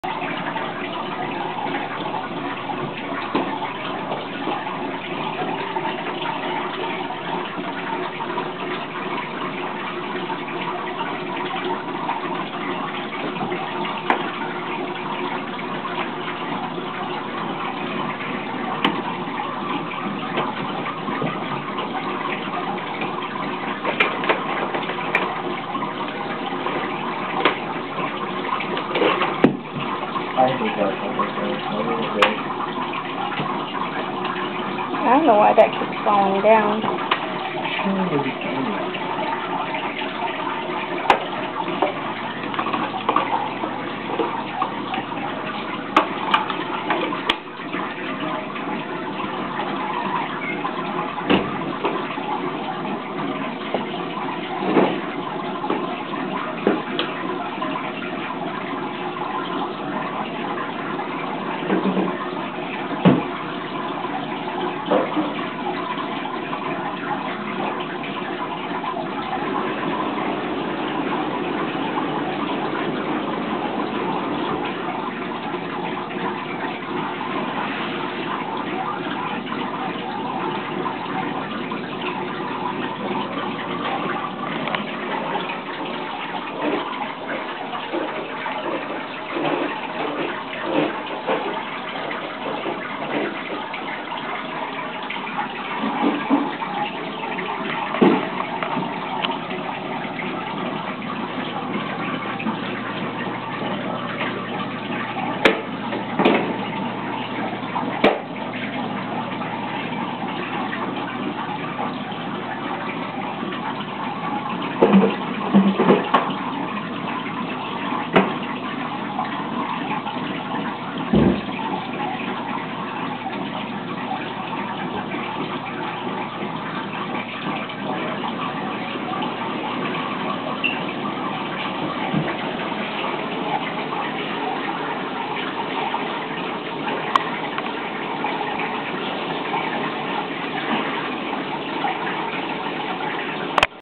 Epsie the Red Eared Slider
This is my awesome turtle, Epsie, with her new 55 gallon tank.